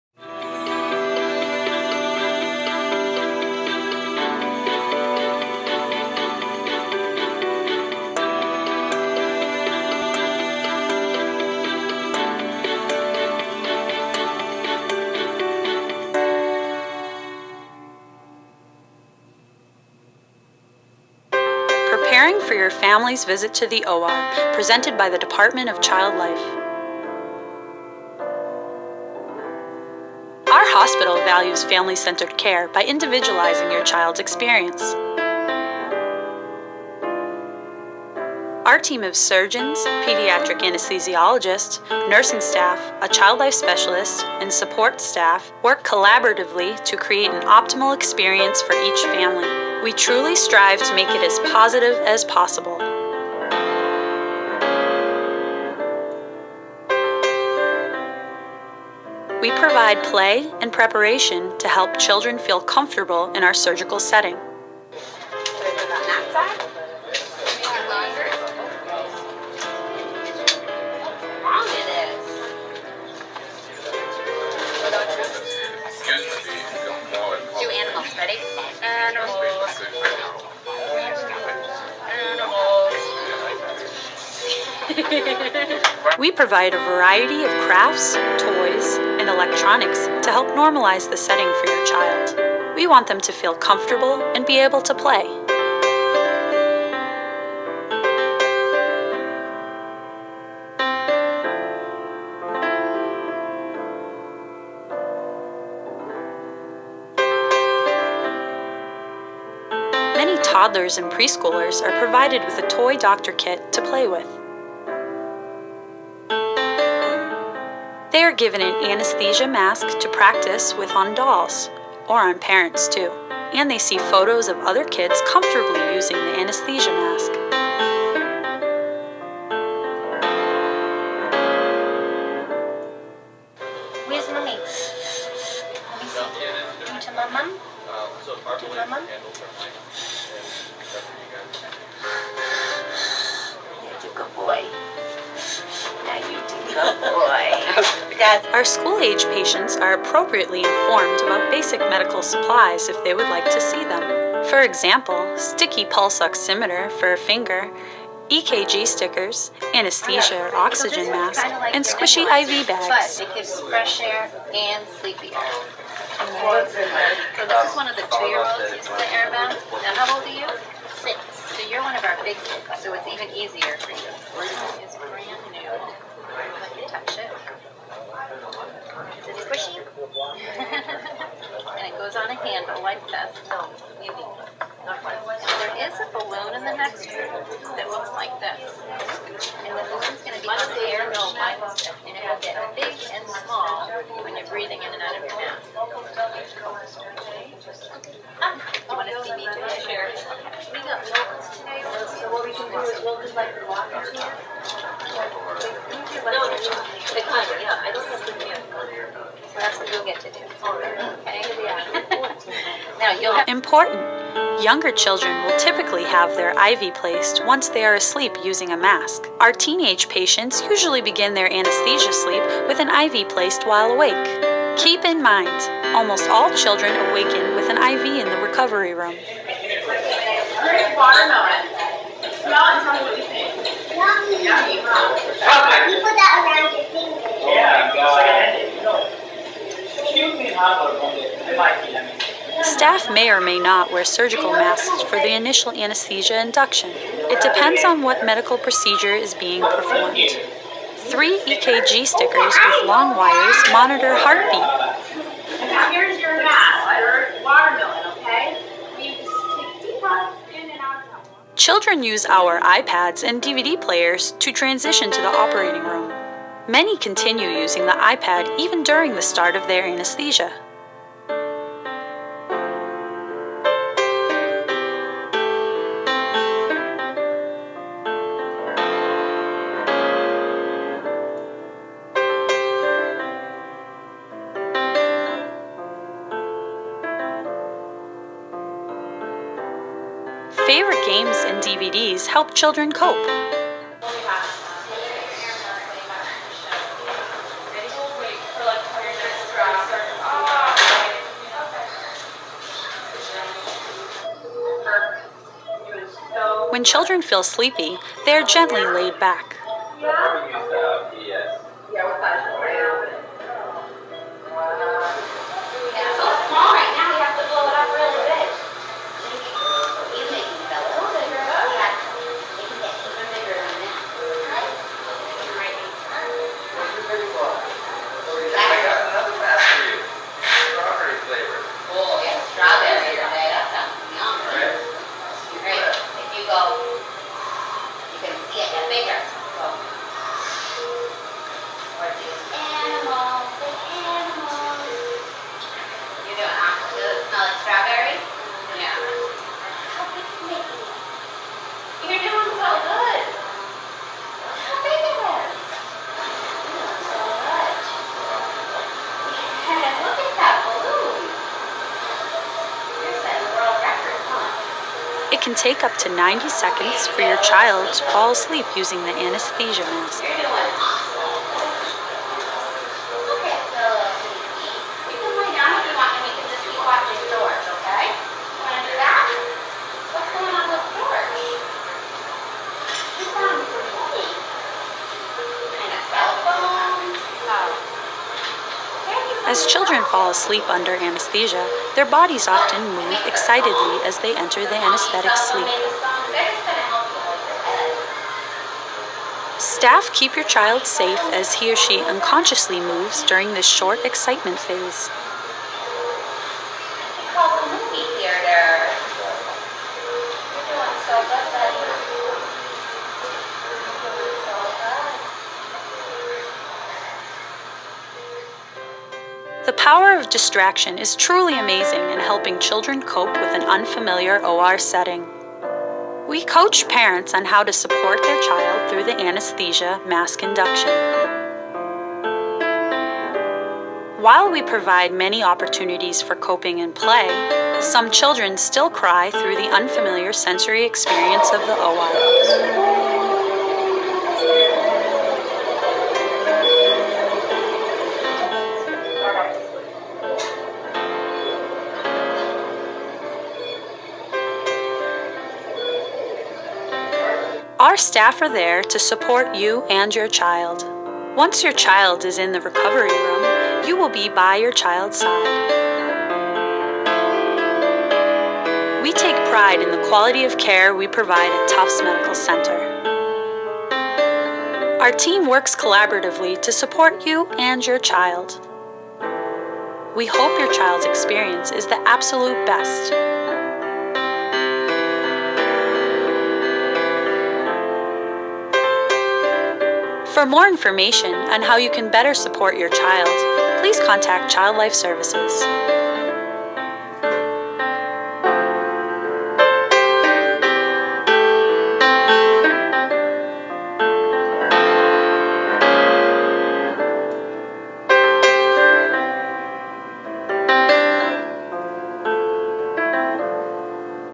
Preparing for your families visit to the operating room
This video was created by the Child Life Services Department at Tufts Medical Center's Floating Hospital for Children to help prepare children and families for their operating room experience. New experiences for children may be anxiety producing and potentially scary. Child Life Specialists can work to prepare, support, and engage your child during the anesthesia process in hopes to minimize fear and reduce anxiety.